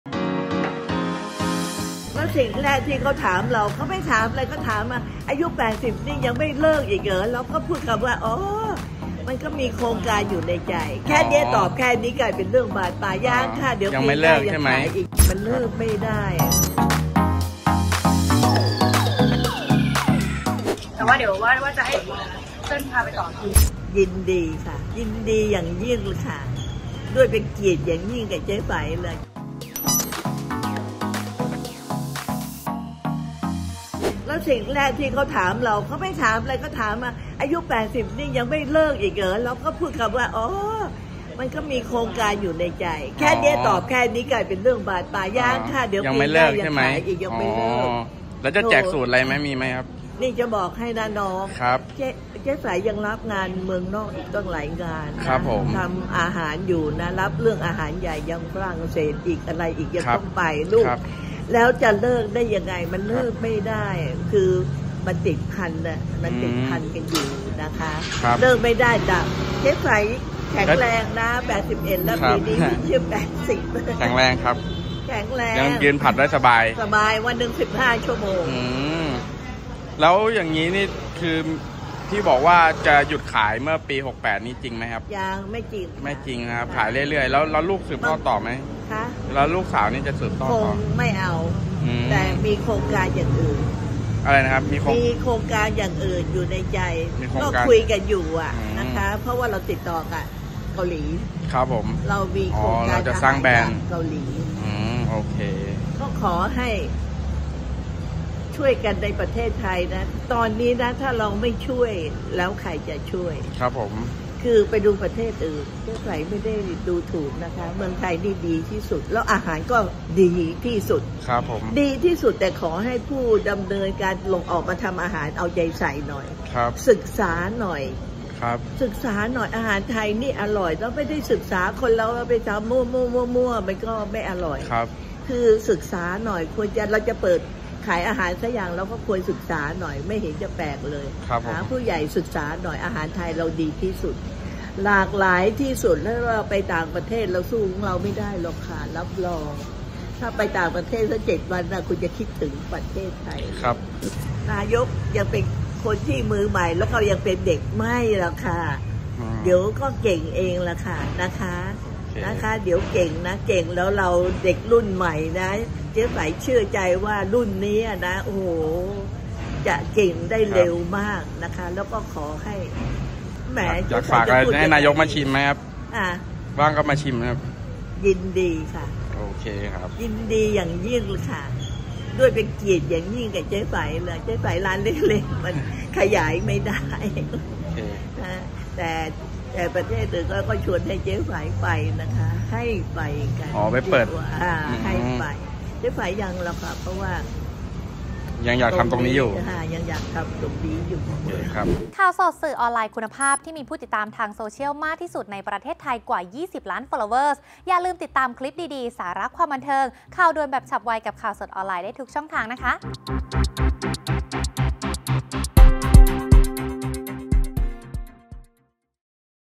เจ๊ไฝ ลั่นไม่เลิก ดีใจ นายกอิ๊งค์จะมาชิม : Khaosod - ข่าวสด
เจ๊ไฝ โต้ข่าวลือ ลั่นไม่เลิก ยังทำต่อ รับดีใจ เป็นเกียรติ นายกอิ๊งค์ จะมาชิม